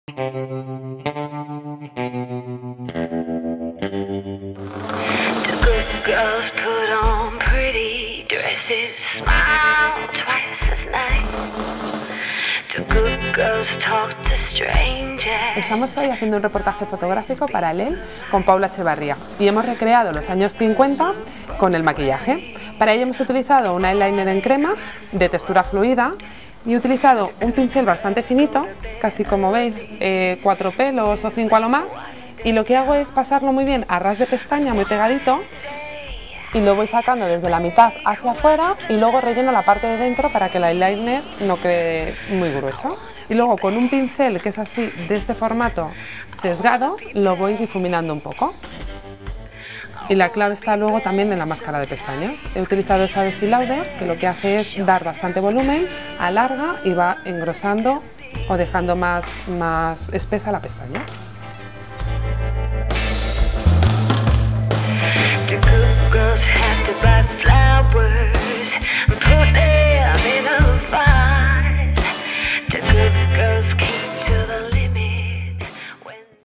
Paula Echevarría: maquillaje retro | Elle España
La maquilladora Sonia Marina nos cuenta paso a paso como conseguir un maquillaje años 50, con una modelo de lujo: Paula Echevarría.